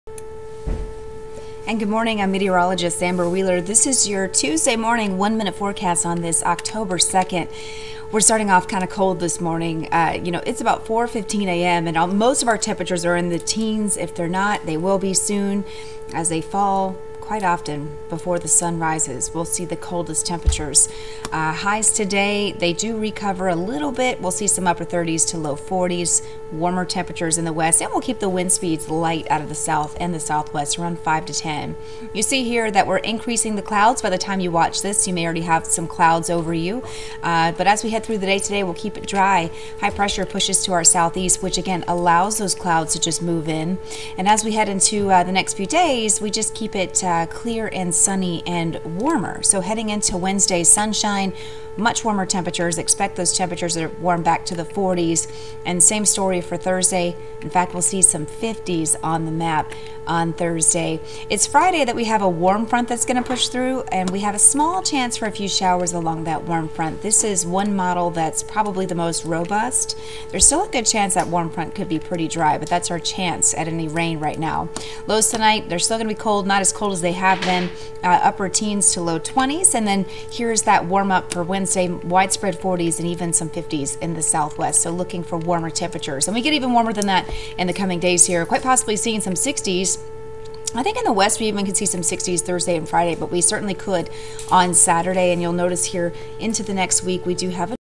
Amber's Tuesday Morning OneMinuteForecast 11/2
Increasing clouds with cool temps.